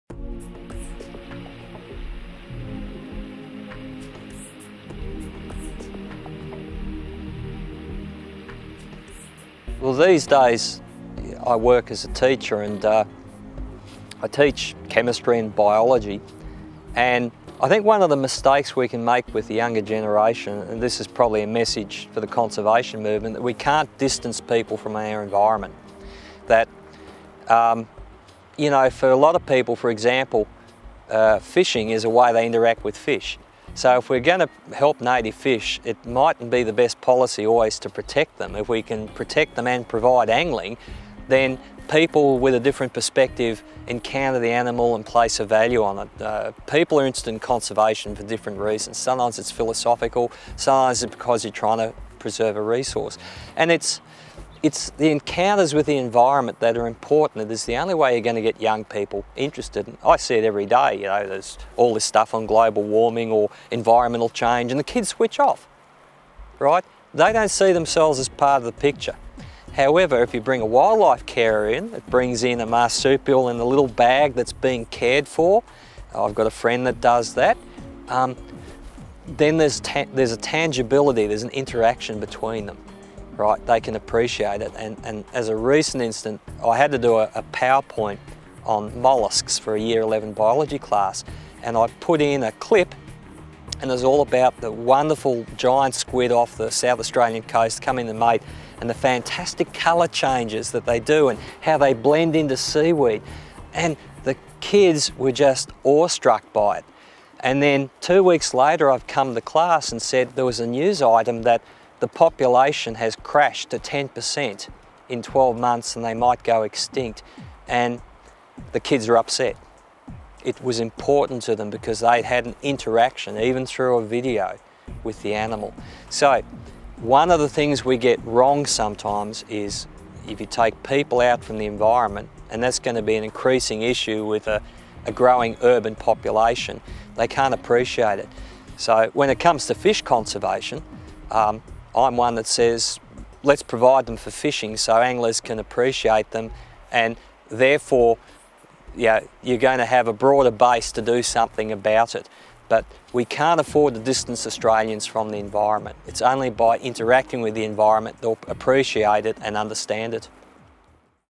Educating the next generation HD
One of the videos created for the Murray-Darling Basin Authority.
Based on the work of Will Trueman - True Tales of the Trout Cod: River Histories of the Murray-Darling Basin.

Further information can be found on the Trout Cod project